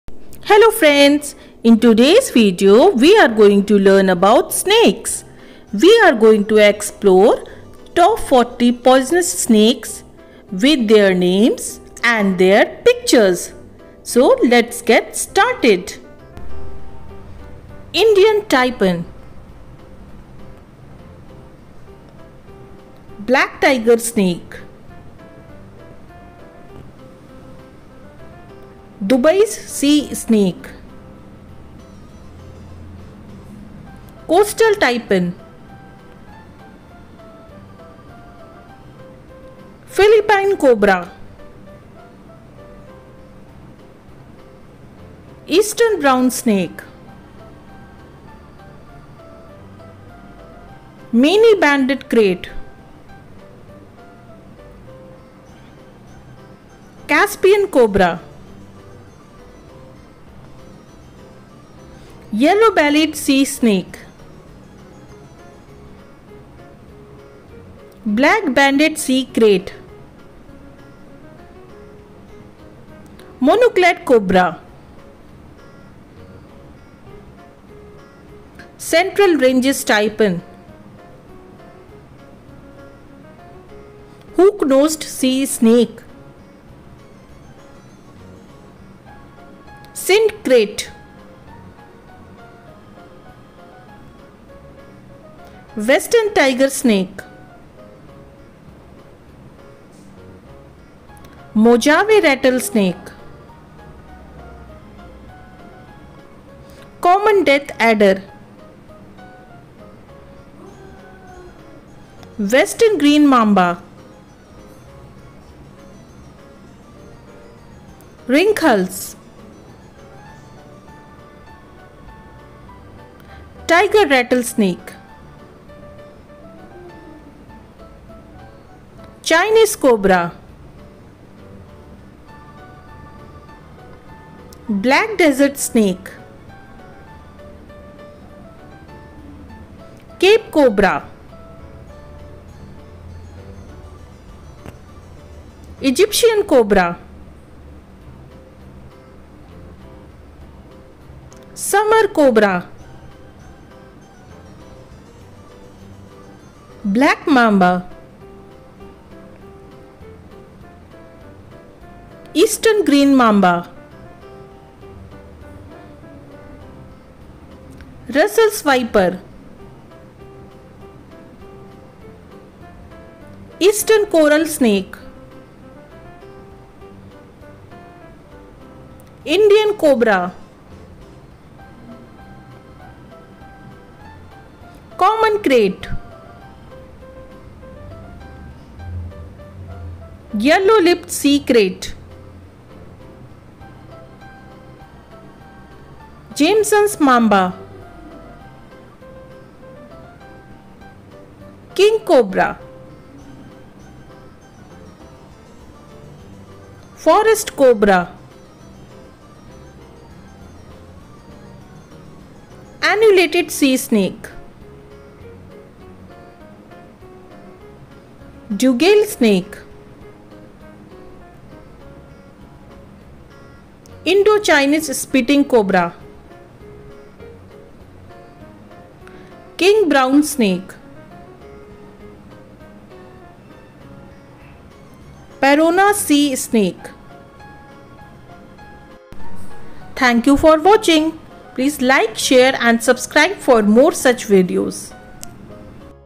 Snakes | Learn Names Of Top 40 Poisonous Snakes In The World With Pictures | Snakes Name
Snakes | Learn Names Of Top 40 Poisonous Snakes In The World With Pictures | Snake's Name | @KidsTutionPoint

Description:

Welcome to our ultimate guide on the top 40 poisonous snakes in the world! In this video, we dive into the fascinating and deadly world of venomous serpents, showcasing the most dangerous snakes on the planet. From the notorious King Cobra to the elusive Taipan, each snake is ranked based on its venom potency, habitat, and risk to humans. We’ve included high-quality pictures and intriguing facts about each species, so you can learn to identify these deadly reptiles. Whether you're a wildlife enthusiast or simply curious, this video is packed with information that could save your life.
Indian taipan
Black tiger snake
Dubai's sea snake
Coastal  tipan
Philippine cobra
Eastern brown snake
Many banded krait
Caspian cobra
Yellow bellied sea snake
Black banded sea krait
Monocled cobra
Central ranges taipan
Hook nosed sea snake
Sind krait
Western tiger snake
Mojave rattlesnake
Common death adder
Western green mamba
Rinkhals
Tiger rattlesnake
Chinese cobra
Black desert snake
Cape Cobra
Egyptian cobra
Samar cobra
Black mamba
Eastern green mamba
Russell's viper
Eastern coral snake
Indian cobra
Common krait
Yellow lipped sea krait ç
Jameson's  mamba
King Cobra ♪
Forest cobra
Annulated sea snake
Dugail snake
Indo Chinese spitting cobra
King brown snake
Peron's sea snake